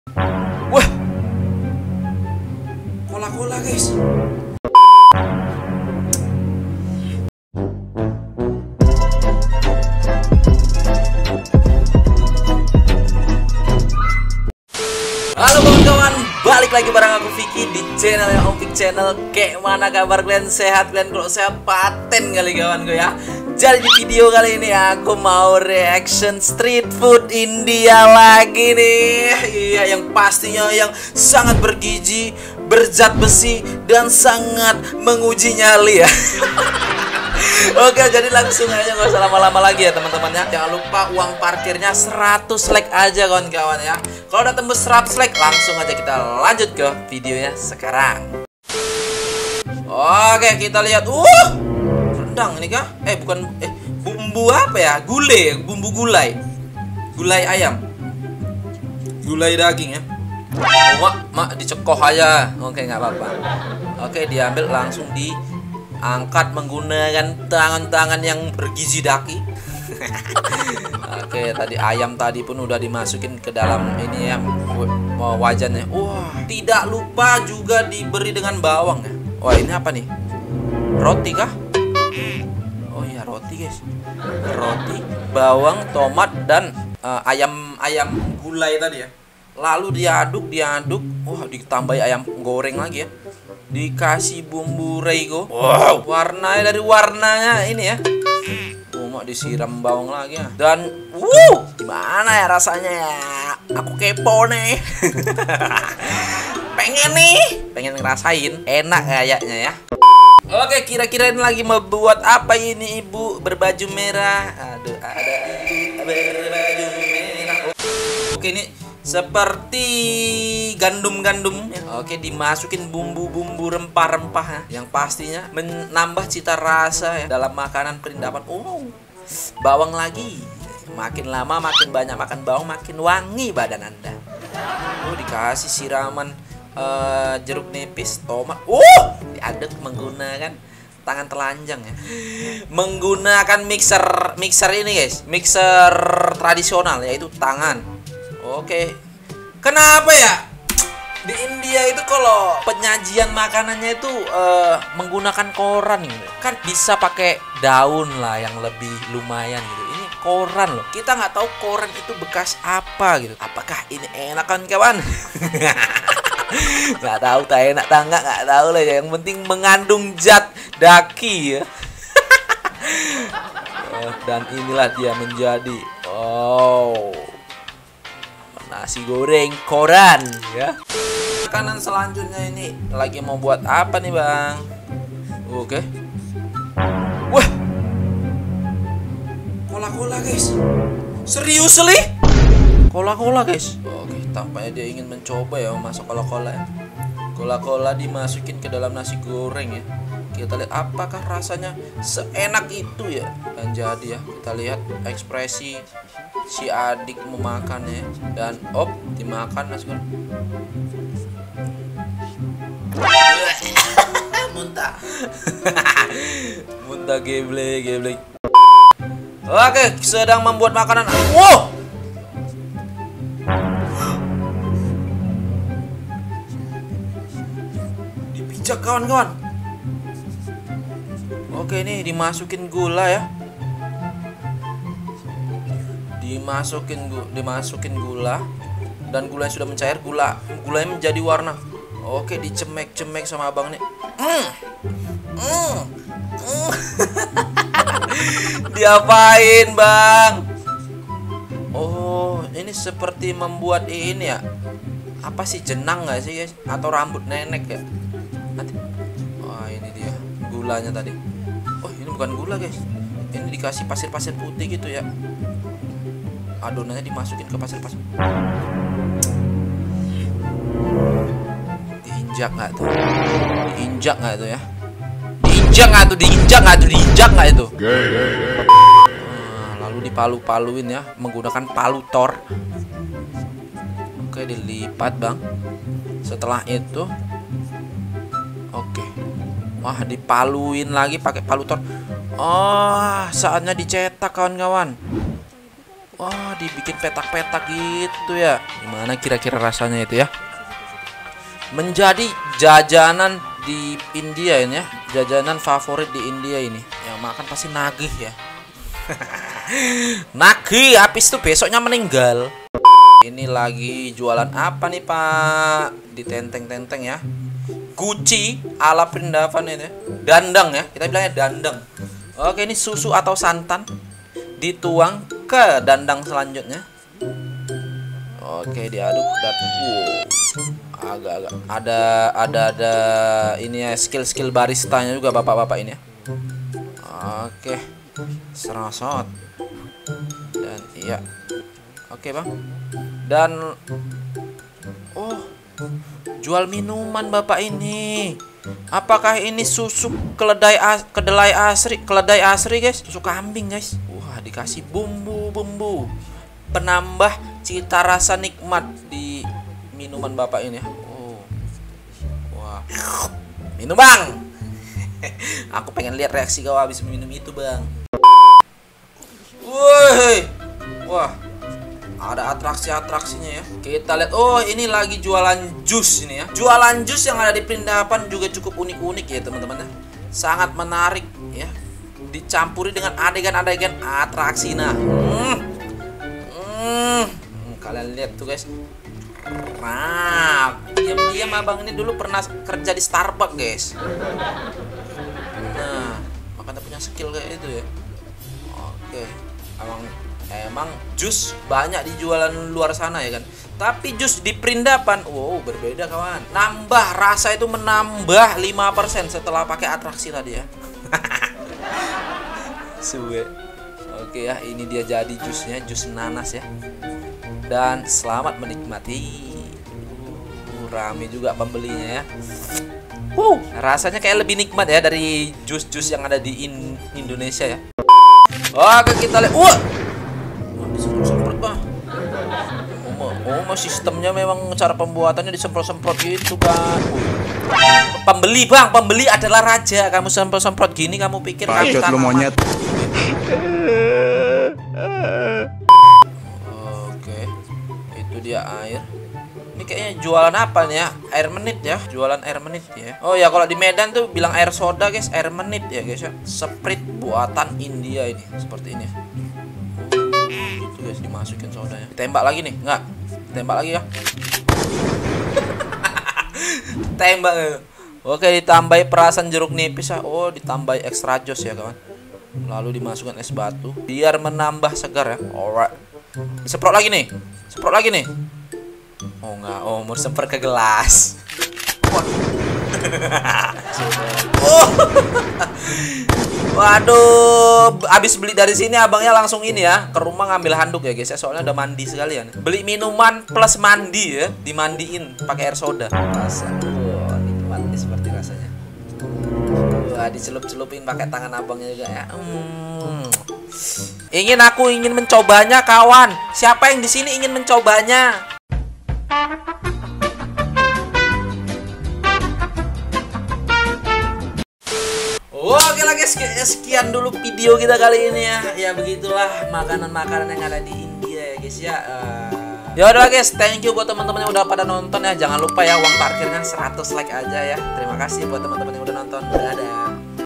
Makanan Viral India, aneh tapi unik ( full reaction )
video reaction street food india lagi nih guys
terimakasih yang sudah dukung terus
cuma niat reaction dan bukan untuk menjatuhkan ya guys
no baper baper yaaa :)